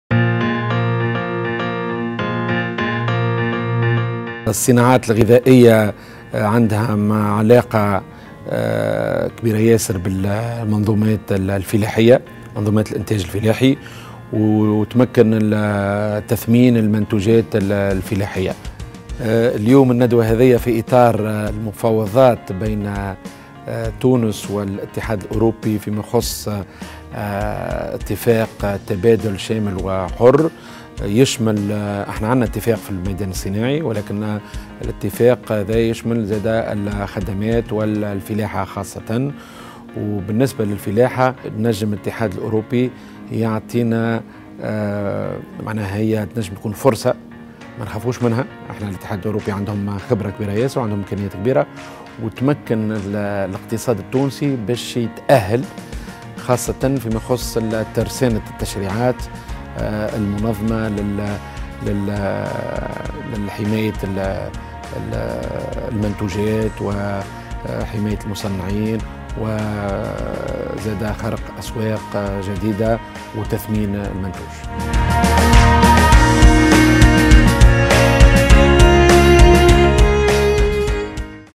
Agriculture et ALECA - Chiheb Slama
Chiheb Slama, vice-président de la fédération de l'industrie agro-alimentaire à l'UTICA Tunisie.

Séminaires Agriculture et ALECA organisés les 13 et 14 septembre 2018 par le Syndicat des Agriculteurs de Tunisie avec l'appui de la Communauté européenne.